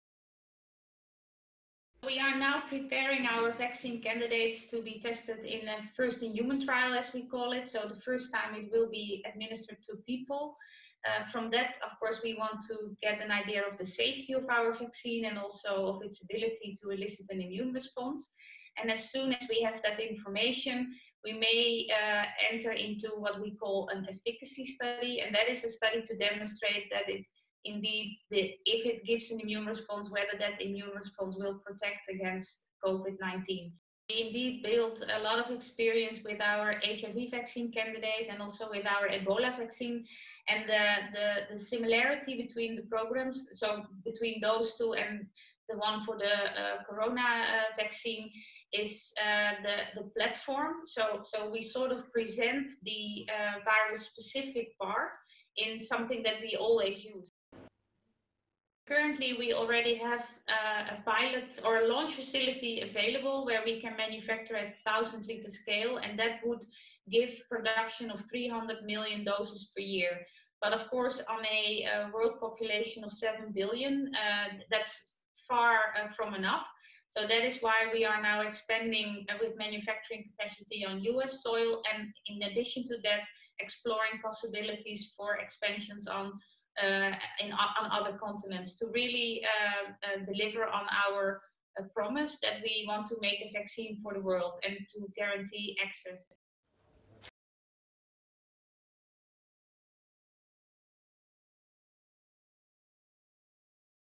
Coronavirus vaccine trial begins in September
Johnson and Johnson has announced that is has selected its lead candidate for a Covid-19 vaccine, after pre-clinical research. Johnson and Johnson and the US Biomedical Advanced Research and Development Authority have committed more than one billion US dollars towards the research and development of the coronavirus vaccine. Johnson and Johnson Global Head of Viral Vaccine Discovery and Translational Medicine, Hanneke Schuitemaker speaks.